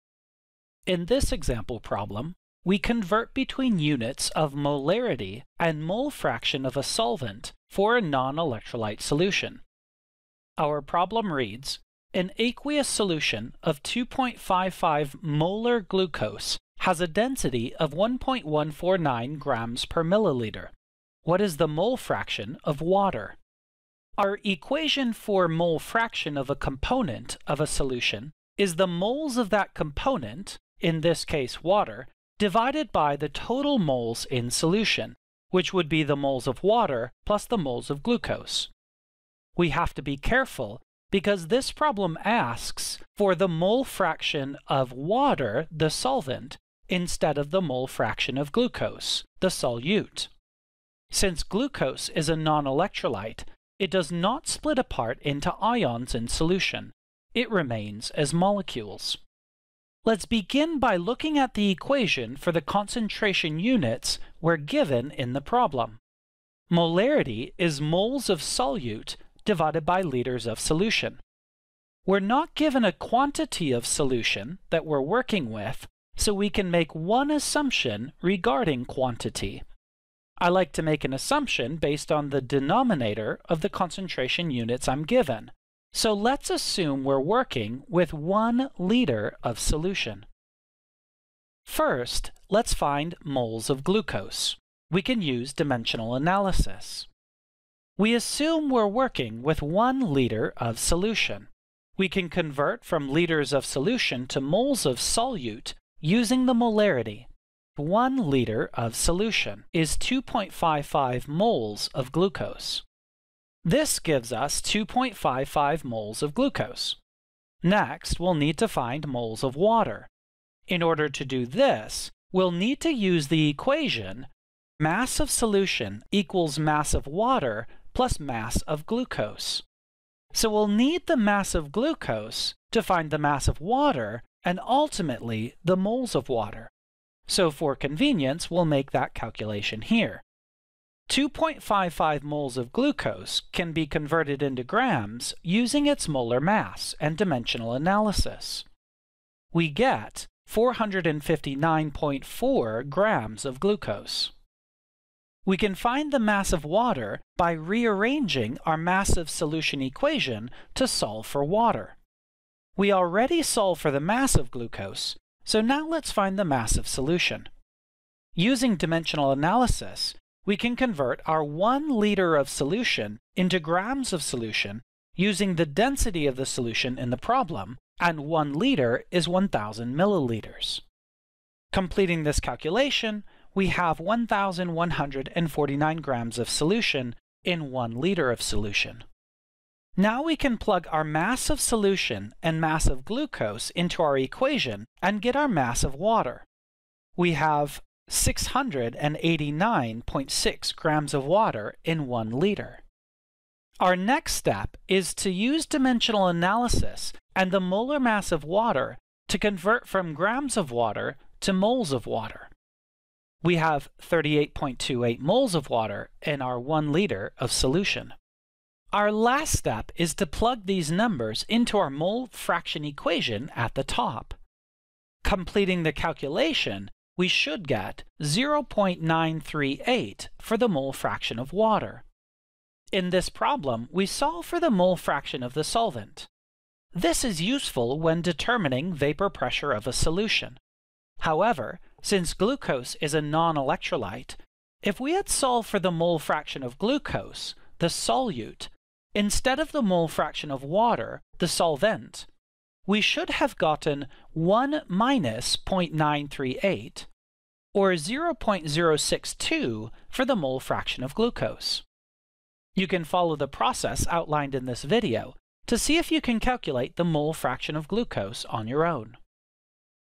CHEM 201: Solutions - Converting between Molarity and Mole Fraction for Nonelectrolyte
In this example, we convert from molarity of glucose to mole fraction of water, the solvent.